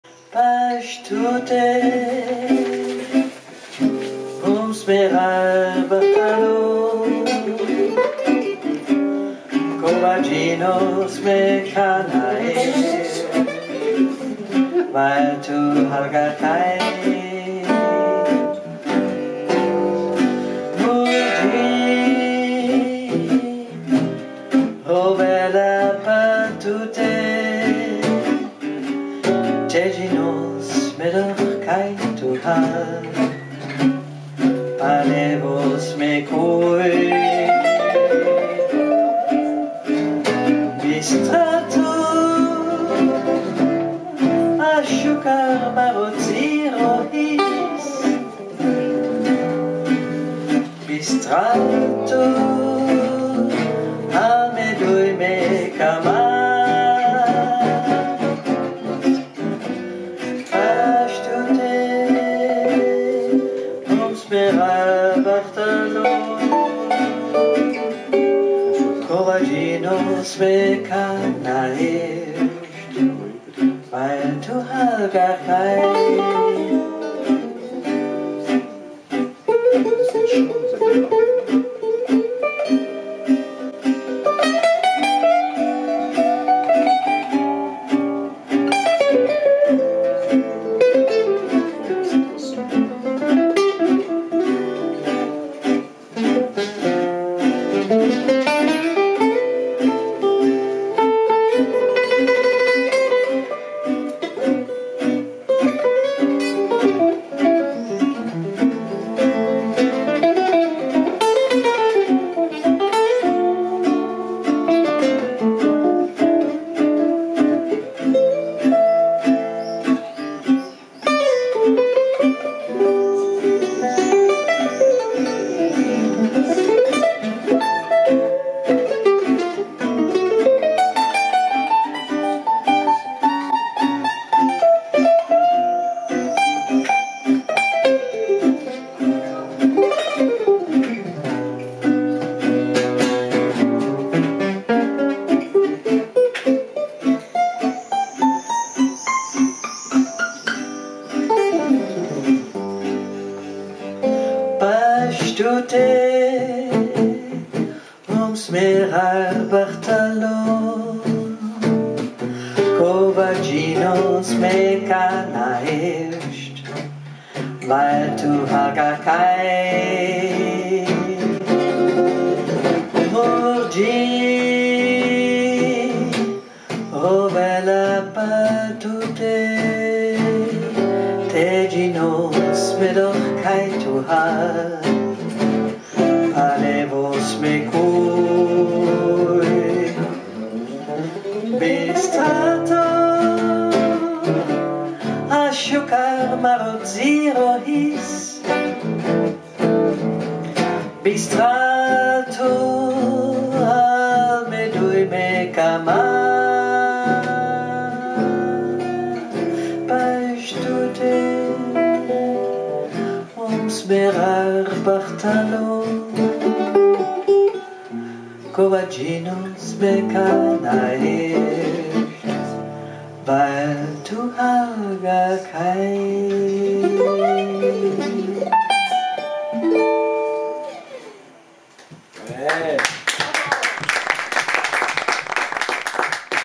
Irene's GJ Adventures 511: Pasch Tute Daniel Givone & Irene
Daniel Givone & Irene at La Petite Maison Utopique in Clisson, France.

July 2018.